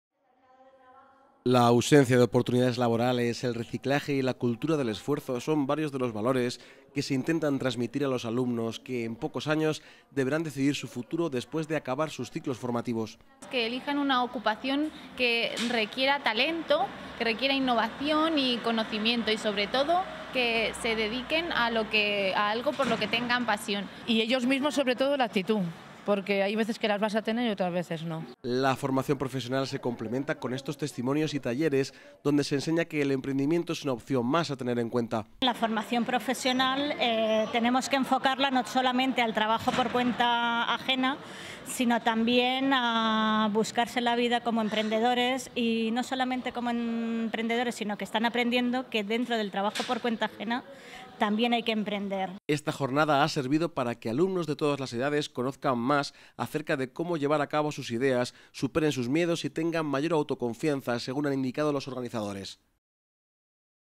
CUENCA IMPARTE TALLERES PARA EMPRENDEDORES FOMENTAR LA CONFIANZA
Favorecer la autoconfianza, combatir la resistencia al fracaso y fomentar la creatividad entre los jóvenes. Ese es el objetivo de un taller de emprendedores que transmite ese espíritu a los alumnos. Se trata de superar miedos y lograr objetivos.